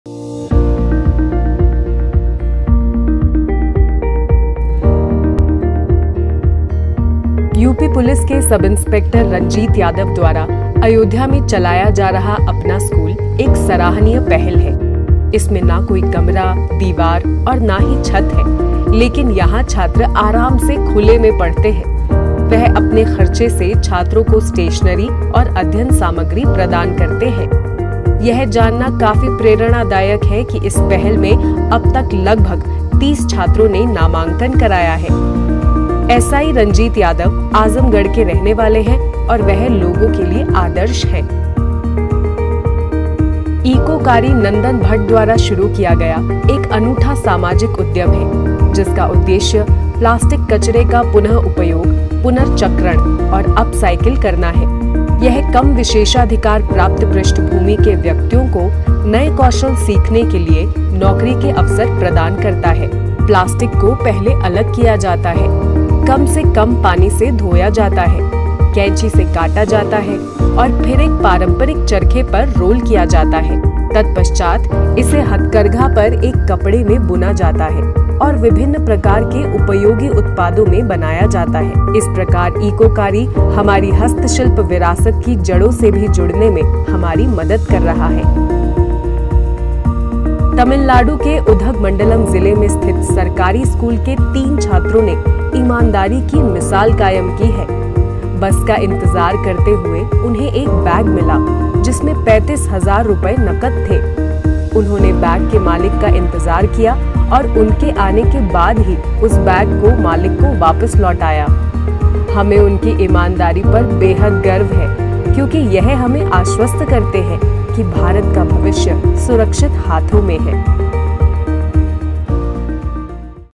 𝑴𝑰𝑩𝒆𝑷𝒐𝒔𝒊𝒕𝒊𝒗𝒆कहानियां जो प्रेरित करती हैं(13 अगस्त 2022)
कहानियां जो प्रेरित करती हैं

🔷एसआई रंजीत यादव द्वारा संचालित 'अपना स्कूल' में छात्रों को मुफ्त शिक्षा का प्रावधान

🔷इकोकारी, प्लास्टिक से उपयोगी उत्पाद बनाने वाला एक अनूठा सामाजिक उद्यम

🔷उधगमंडलम के 3 छात्रों की ईमानदारी की कहानी